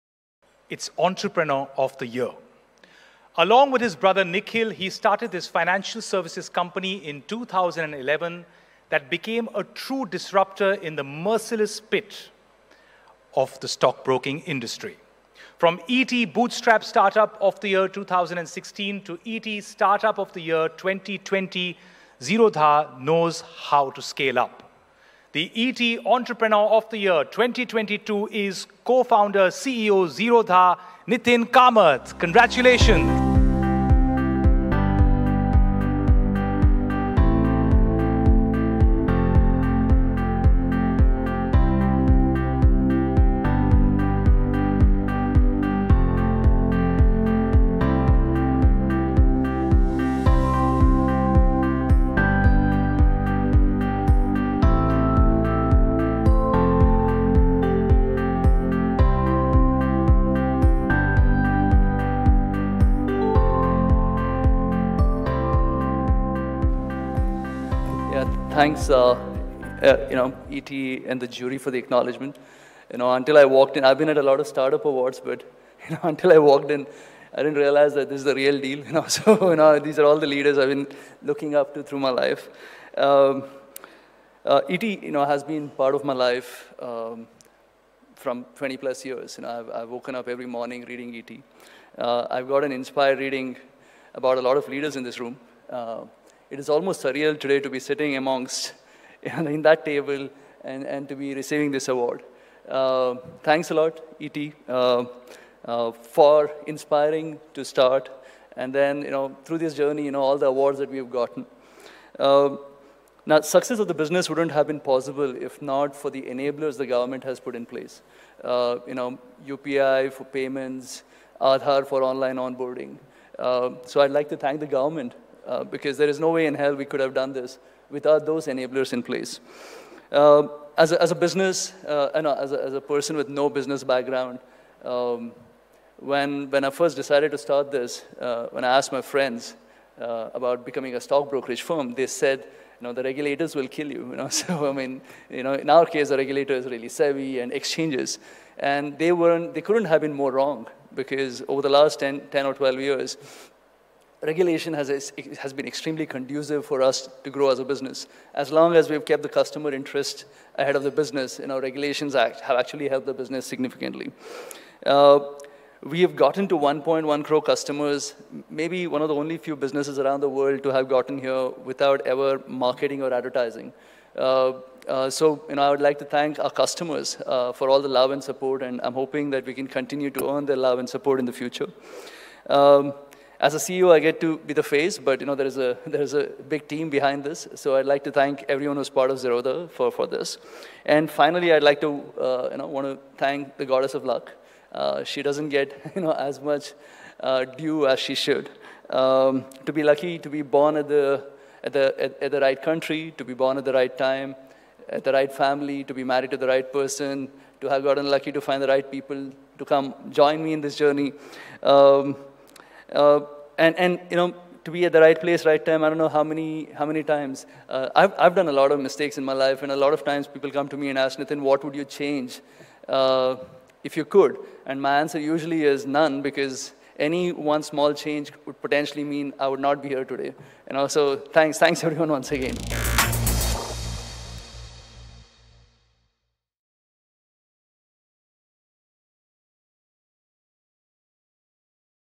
ET Awards 2022 | Entrepreneur of the Year: Nithin Kamath, Founder, Zerodha
Nithin Kamath, Founder, Zerodha, wins the Entrepreneur of the Year Award at The Economic Times Awards for Corporate Excellence. Kamath and his team at Zerodha realised the need for a more affordable and efficient platform for day traders, making them a significant player in the highly competitive stockbroking industry.